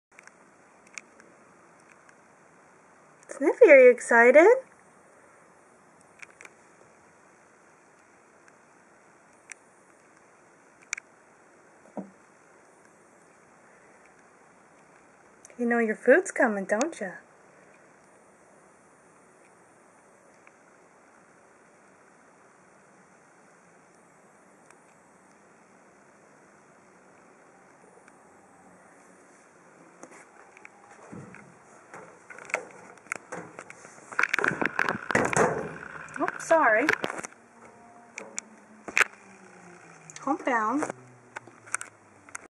sniffie goober
sniffie was very excited for his food